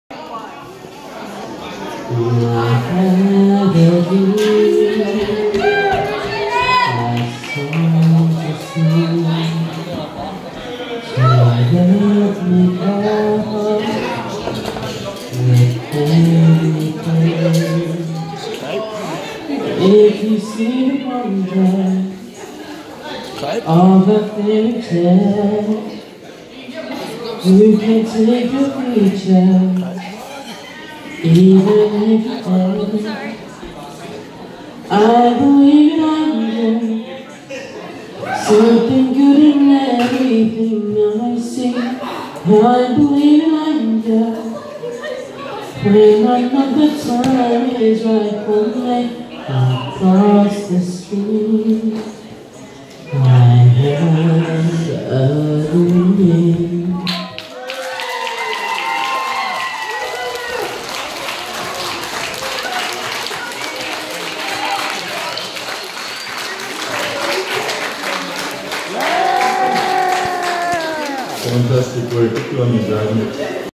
The singing Cliquer!!
Look at this amazing friend of mine who is singing great at a Friday night disco. He is magnificent and I think he has good talent :-D

Filmed on Friday September 21, 2018